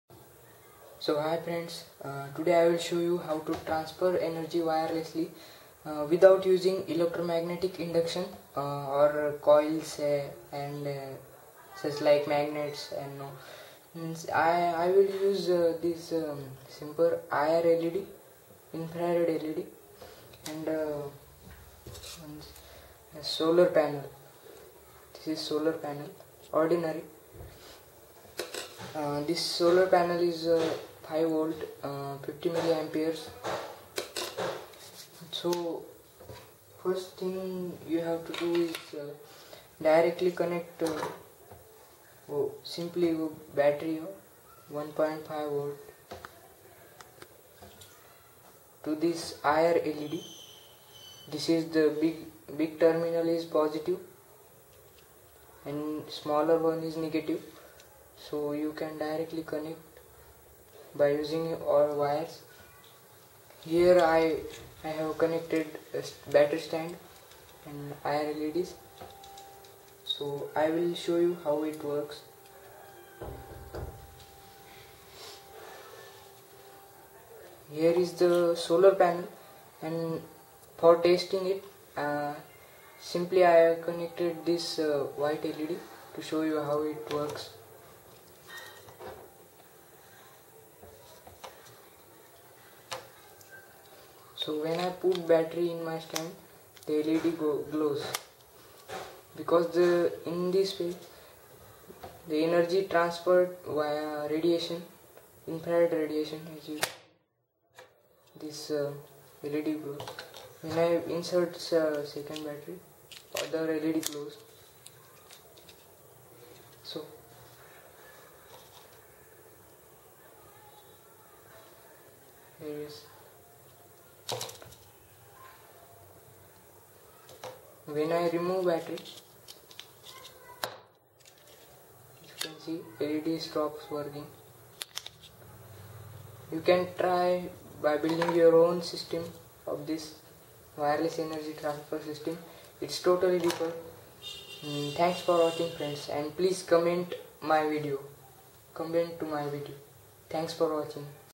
Wireless Energy Transfer
There are many videos on youtube about energy transfer but in this video totally new phenomenon is used to transfer energy using infrared led as sender & solar panel as an power receiver.